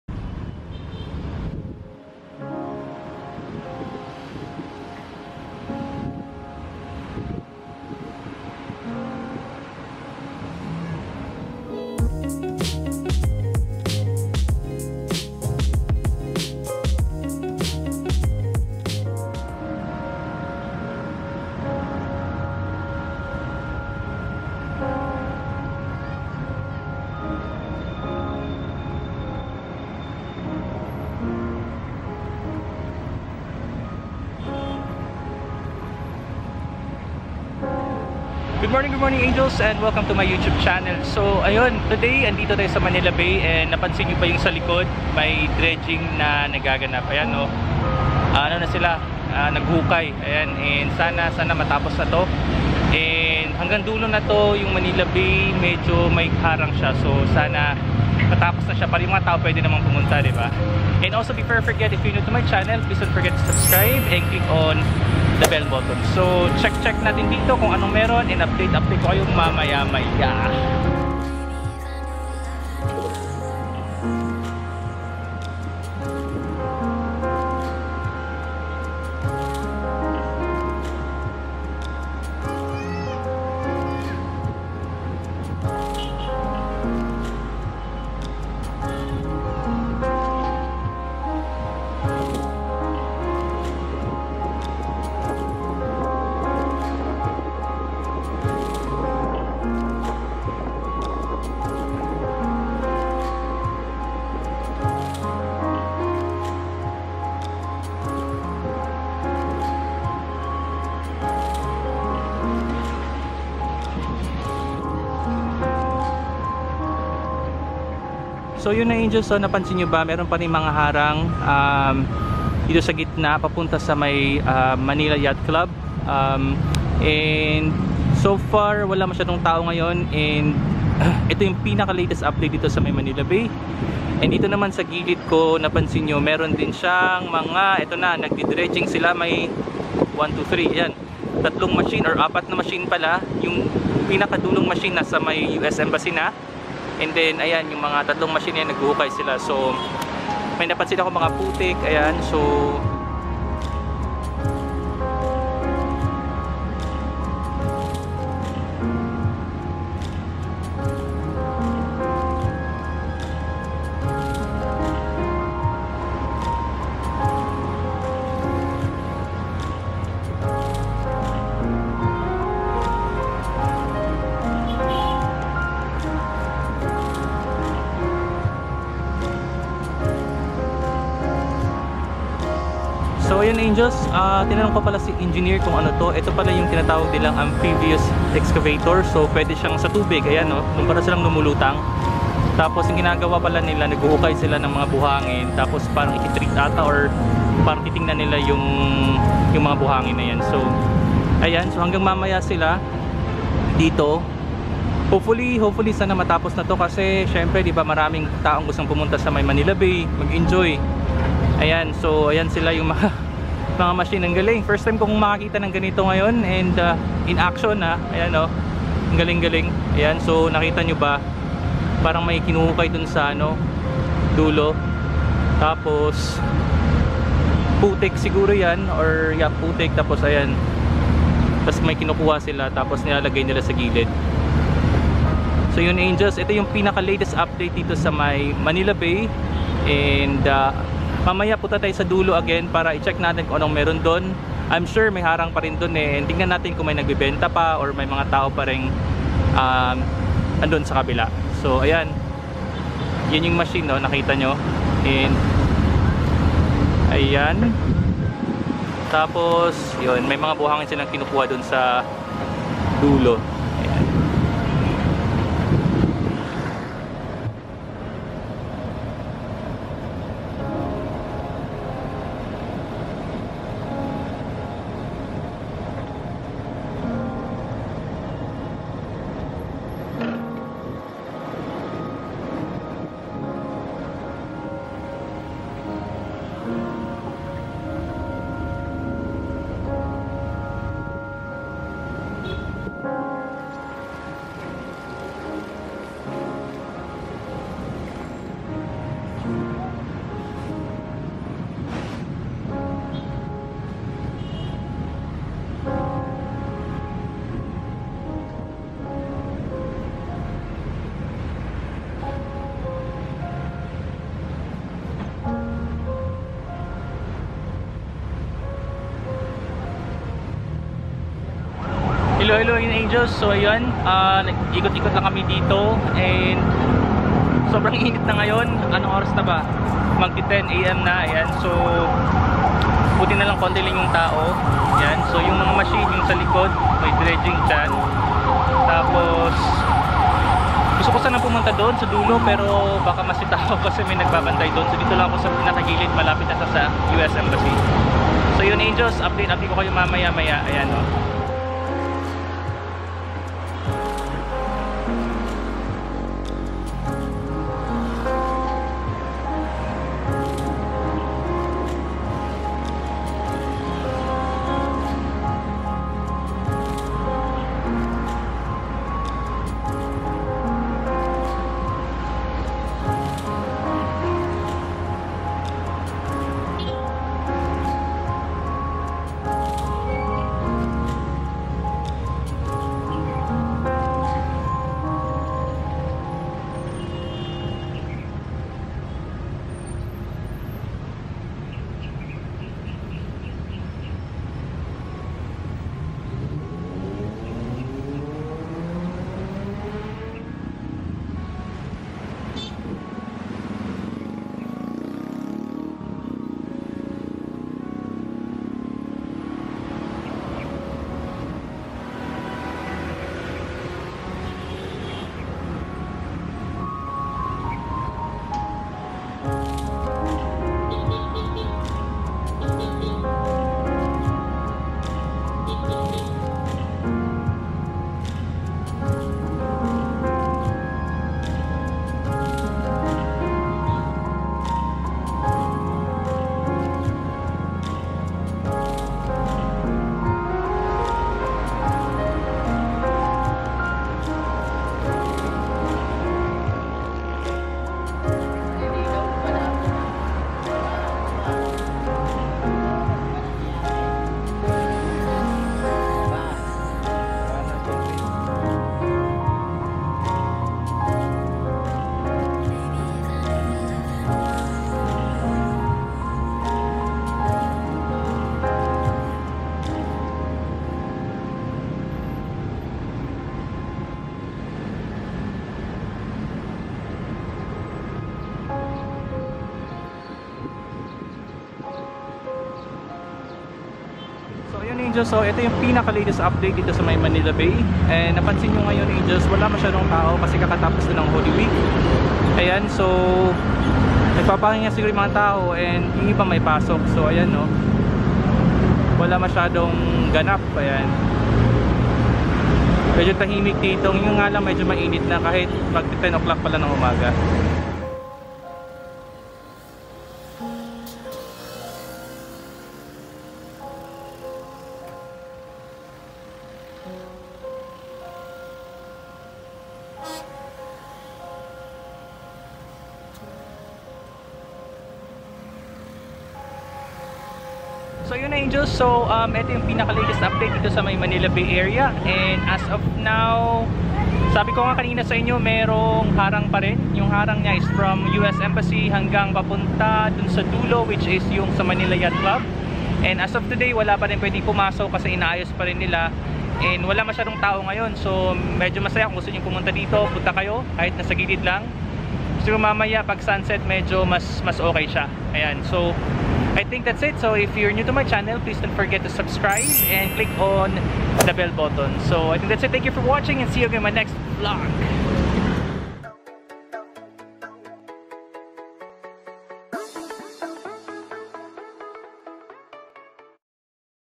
DREDGING IN ACTION || MANILA BAY MAS LALO PANG GUMANDA || vlog# 54
Earlier this month, the Department of Public Works and Highways started dredging operations to remove thick layers of silt and garbage that had gathered on the sea floor of Manila Bay which had accumulated over the years & had reached a depth of 4 meters, or over 13 feet.

I ❤️ 🐕 🏊 🏃 ✈️ 🌏

New 📽 every Monday, Wednesday & Saturday @ 6pm (🇵🇭 time)

✏️ Editing Software:
• iMovie

™ CharlieMagne24/7